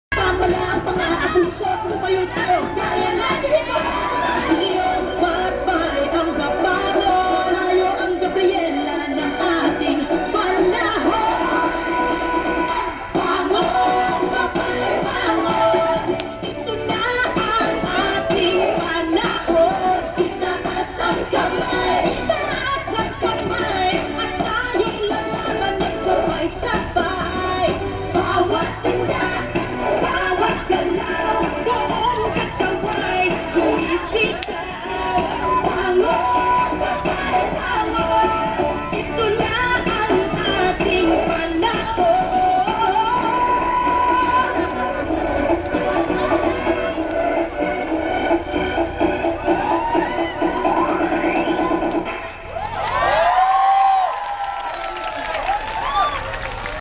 Part of the International Women's Day Program in UP Diliman
To show support during the International Women's Day activities in UP Diliman, the men take front row for this round of the One Billion Rising dance. UP Diliman, Quezon City. 8 March 2013. APDV0520